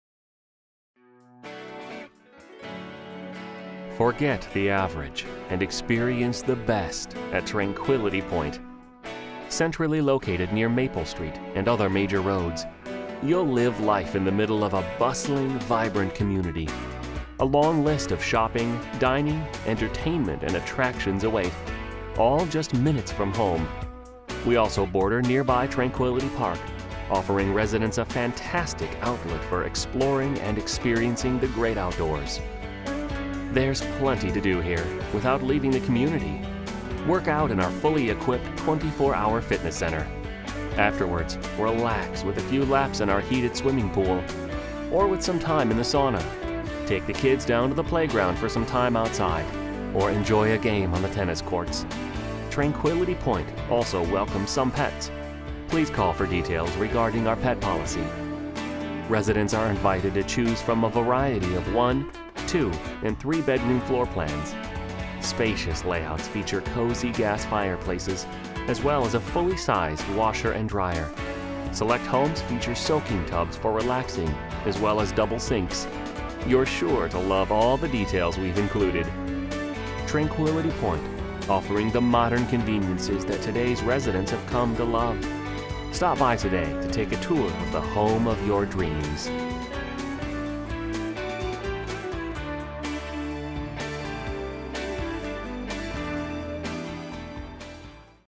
Tranquility Pointe-Omaha Apartments
Omaha Apartments-Tranquility Pointe is a luxury apartment community designed for those who want a home reflecting their discriminating style and tasteful elegance, located in Northwest Omaha, Nebraska. Our huge one bedroom, two bedroom and three bedroom apartment homes for rent feature full size washers and dryers, extra large closets, incredibly large bedrooms and cozy gas fireplaces. Bordering beautiful Tranquility Park, our apartment community reflects the careful attention given to detail while offering the convenience of abundant amenities. Peaceful surroundings, privacy and accessibility define the lifestyle available. Enjoy the luxury of a dine-in kitchen, complete with breakfast bar and space-saver microwave. Work out in our 24-hour fitness center, equipped with free weights. Finish your workout with relaxation in our heated swimming pool and sauna. Tranquility Pointe, offering you convenience, elegance and class.